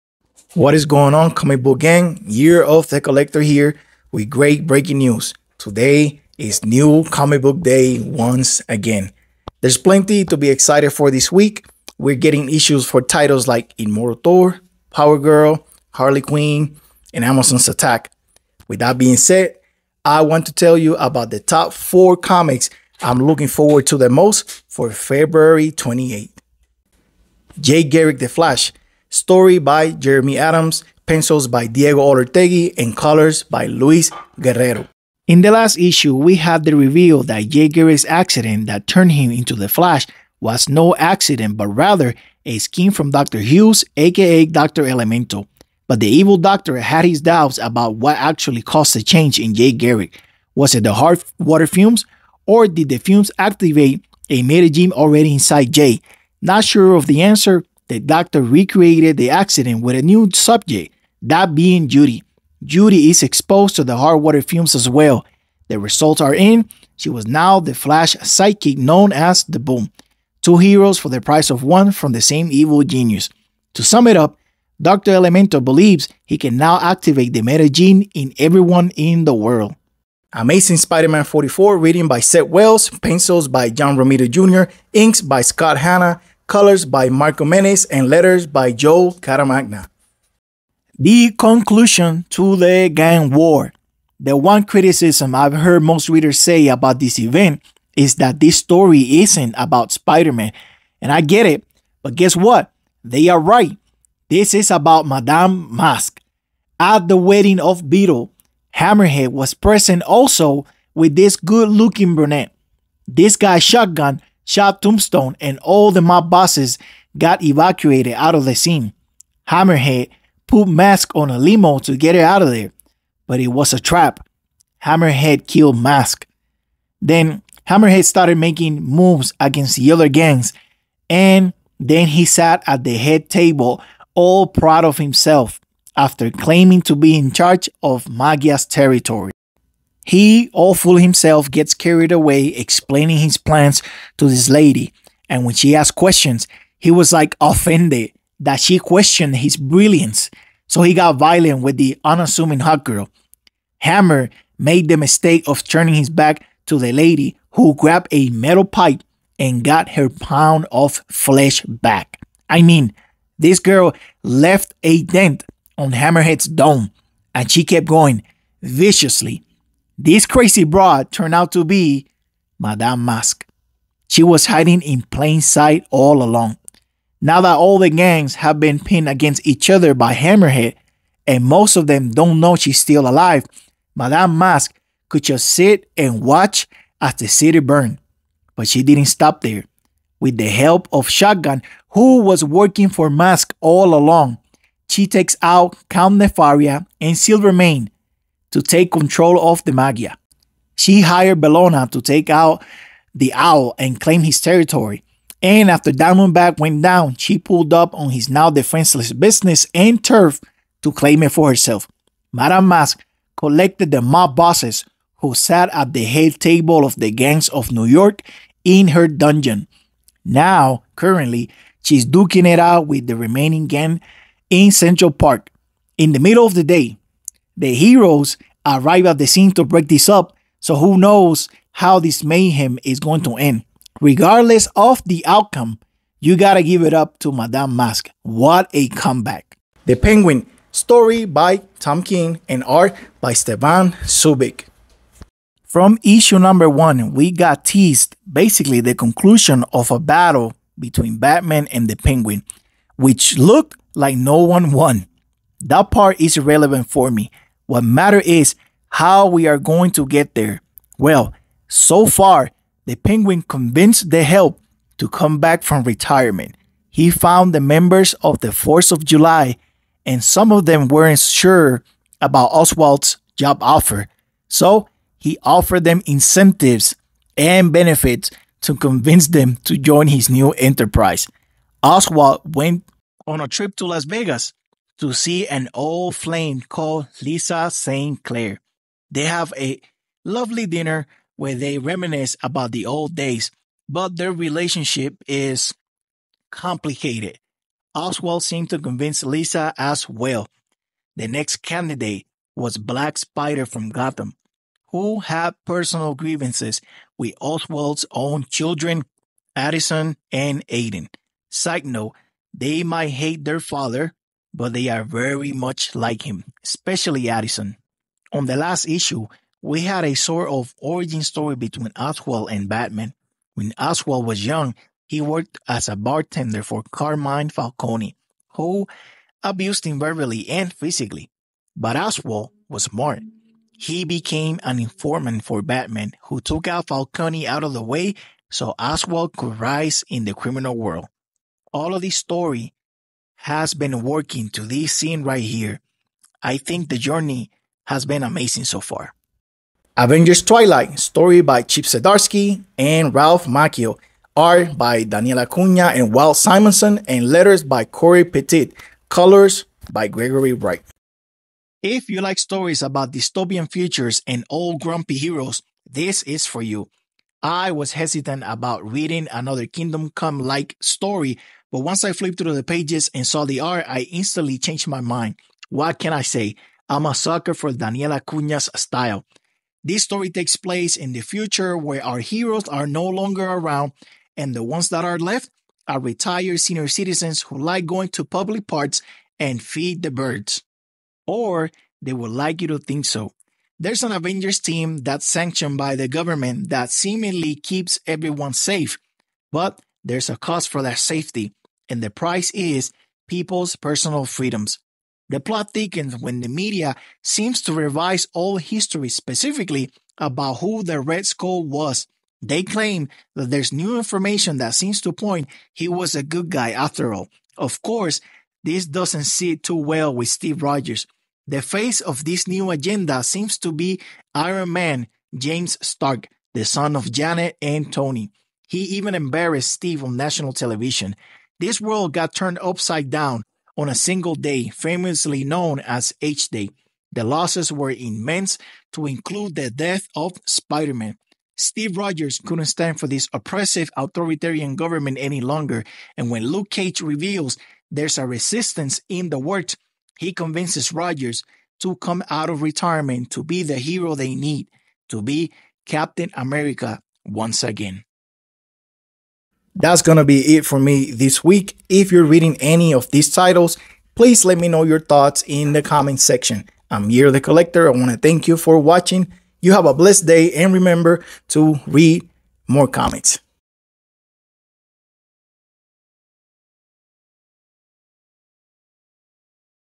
My Comic Book Pulls: Feb 28 (Top 4 Titles)
Comic Pulls for this week:
0:00 Intro
0:27 Jay Garrick: The Flash
story by Jeremy Adams, pencils by Diego Olortegui & colors Luis Gerrero.
1:20 Amazing Spider-Man
written by Zeb Wells, pencils by John Romita Jr., Inks by Scott Hana, colors by Marcio Menyz and letters by Joe Caramagna
3:59 The Penguin
story by Tom King and art Stevan Subic
5:49 Avengers Twilight
by story by Chip Zdarsky and Ralph Macchio, art Daniel Acuña and Walt Simonson, letters Cory Petit and colors by Gregory Wright